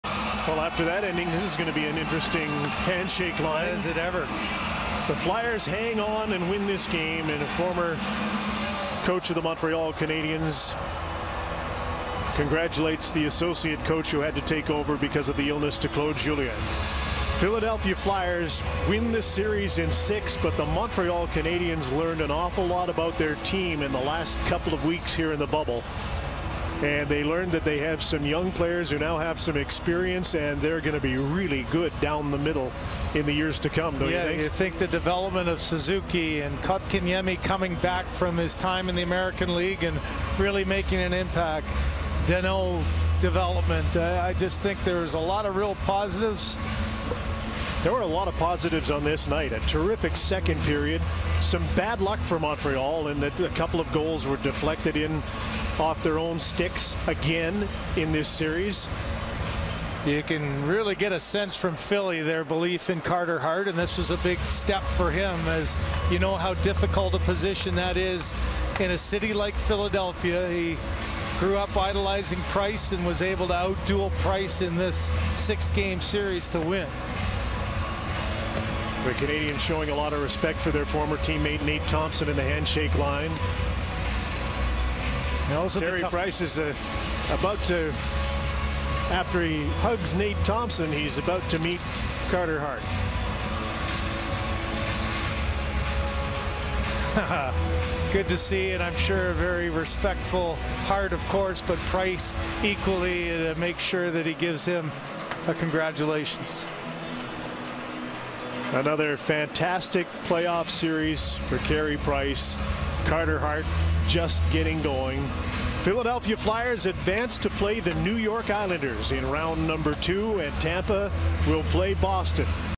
Montreal Canadiens And Philadelphia Flyers Shake Hands After Spirited Series
The Montreal Canadiens and Philadelphia Flyers shake hands after the Flyers eliminated the Canadiens in six games.